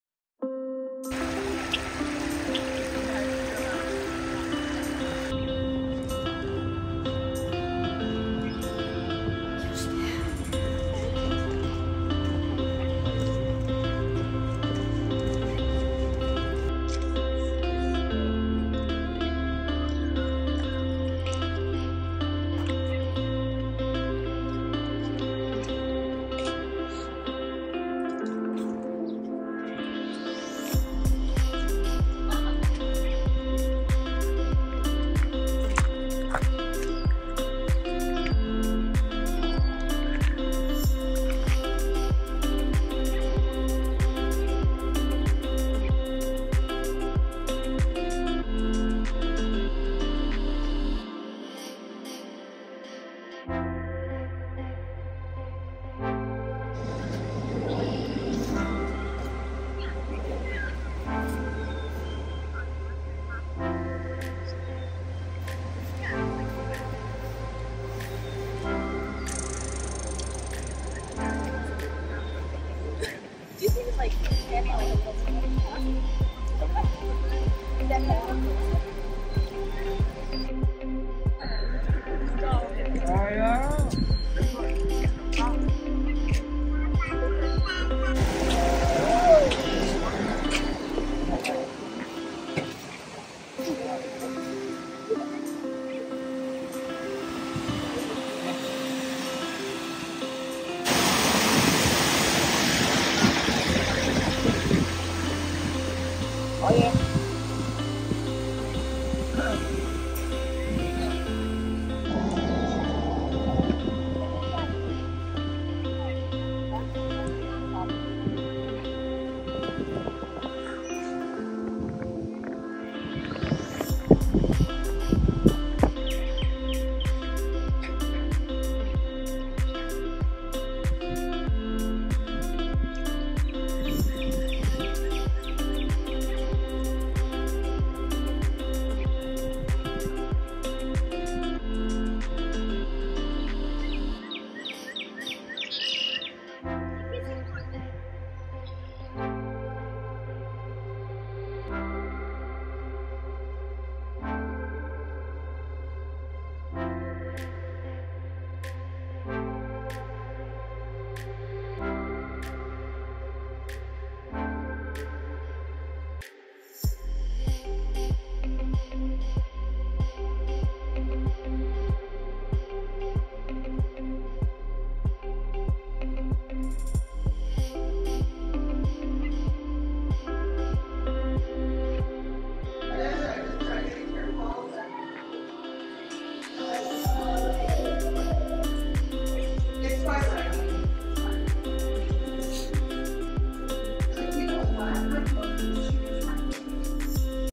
Best park to visit to see Cherry Blossoms in Toronto - Edward Garden
Best park in Toronto to visit in summer or after winter to see cherry blossoms